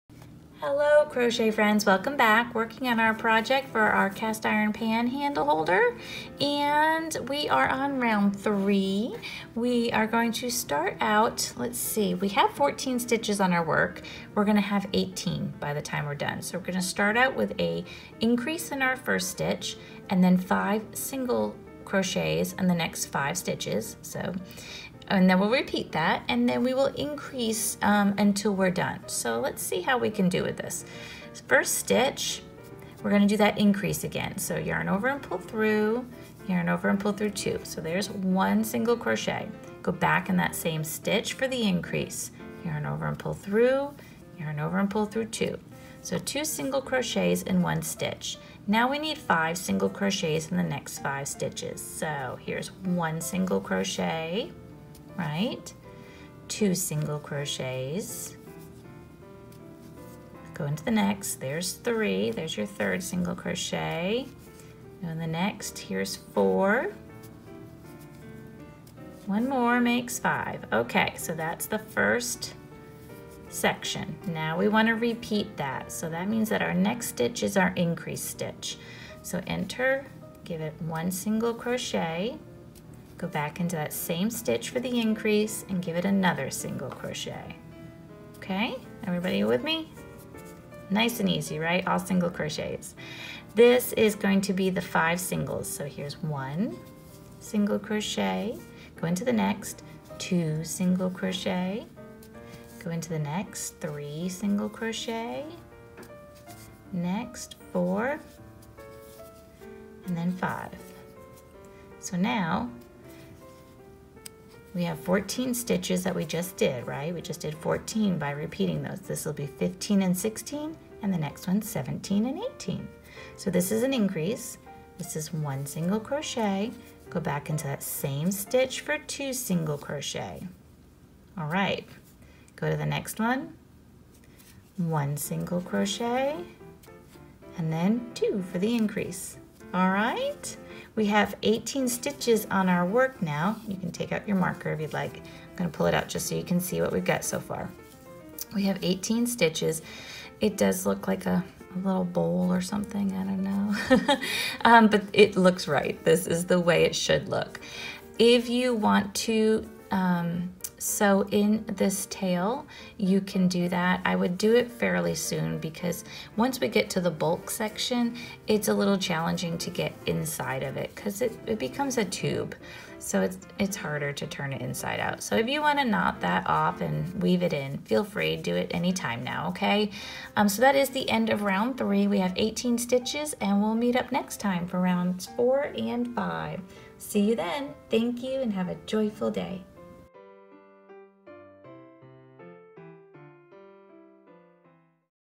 Easy Crochet Pan Handle Holder Round 3
You will need:
worsted weight cotton yarn
G/4mm crochet hook
yarn needle
scissors

Round 3:
*inc in 1st st, sc in next 5 sts  *twice
inc in next 2 sts (18 sts)